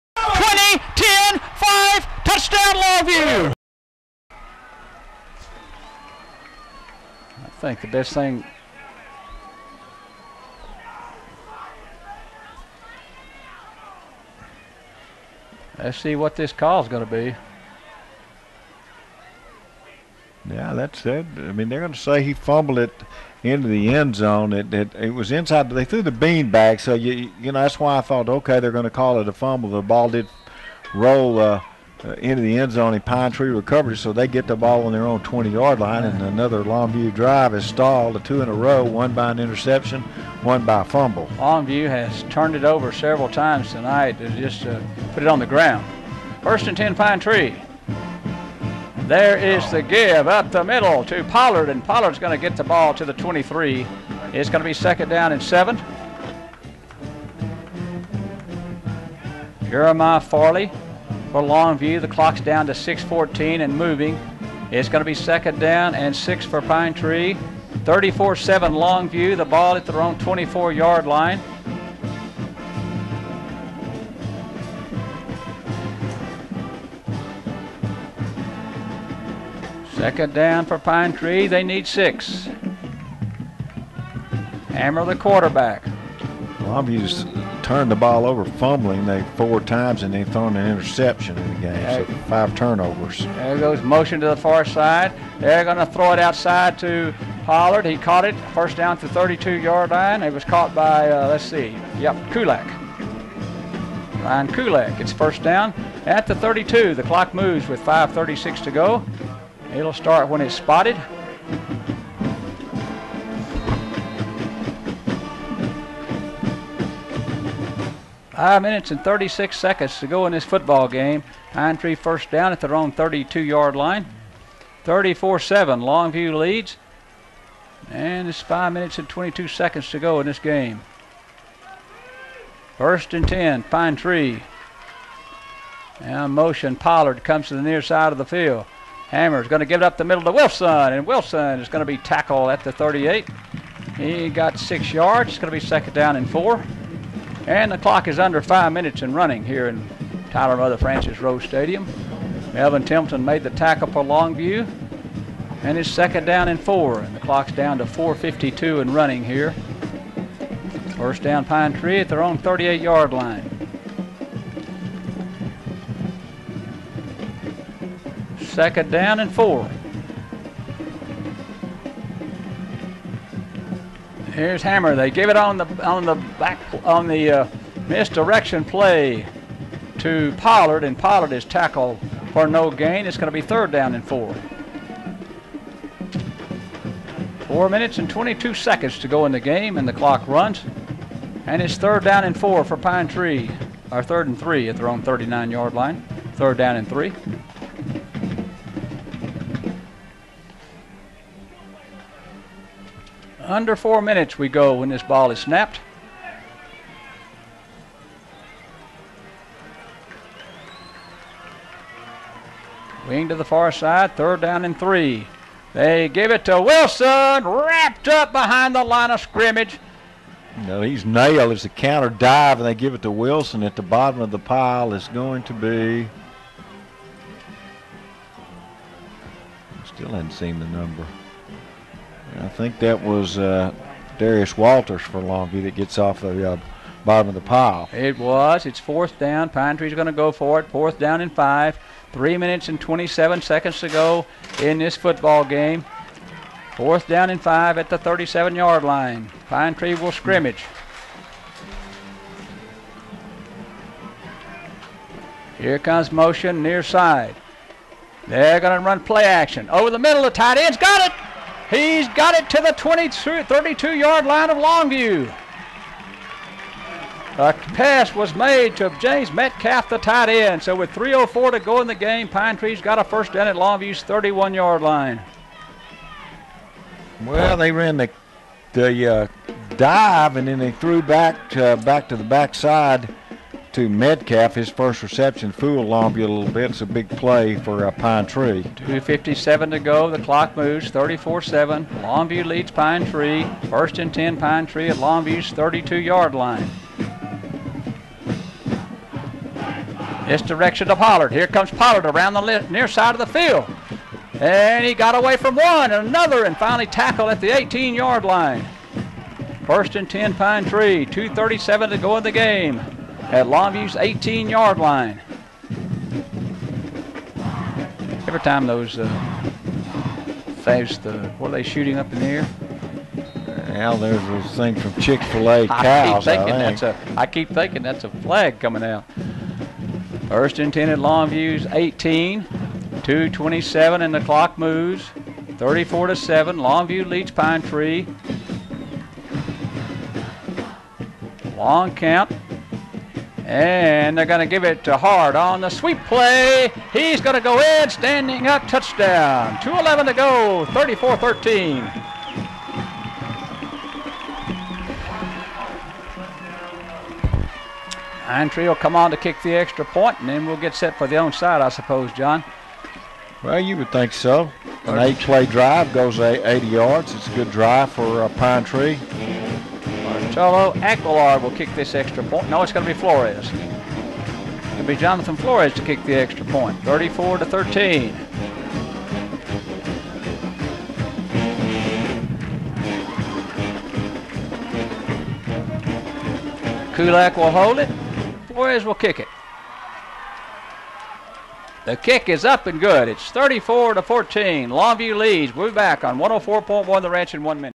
Longview vs Pine Tree, 2008, 4th Quarter Part 2
Despite not meeting for 21 years, the Lobos' and Pirates' cross town rivalry took no time at all at refinding that old hatred they showed throughout the 80's.

Longview, looking to complete it's 5th straight undefeated district campaign, strolled into Tyler's Rose Stadium with a 7-2 record (8-1 on the field) and a 27 game district winning streak. The Pirates however were 2-6 on the season and looking for an upset.

The Pirates put up a good fight for the first 2 quarters, causing the Lobos to fumble it 4 times on the night, but in the end...Longview showed why they hadn't lost in district in 5 straight years. Longview knocks off cross town rival, Pine Tree, 41-14.